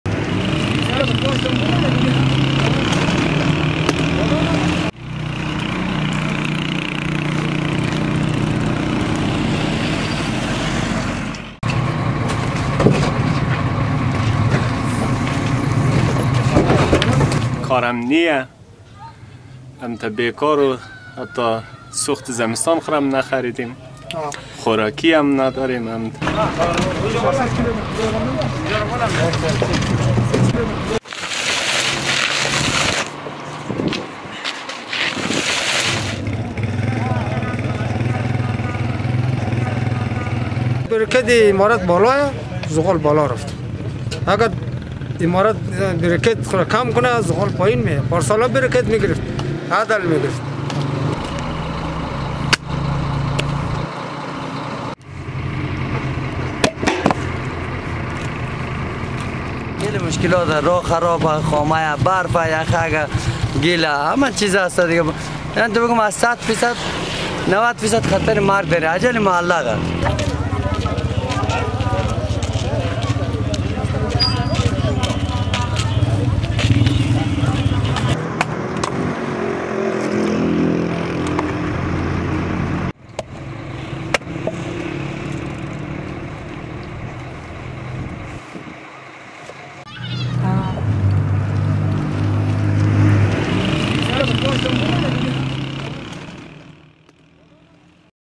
Afghans Struggle To Heat Homes Amid Soaring Winter Fuel Prices
Many people in Afghanistan's central province of Daikundi are struggling to heat their homes this winter. Coal is one of the most common fuels but has become hard to find in the provincial capital, Nili. High taxes and transport costs are some of the main causes for the rising fuel prices.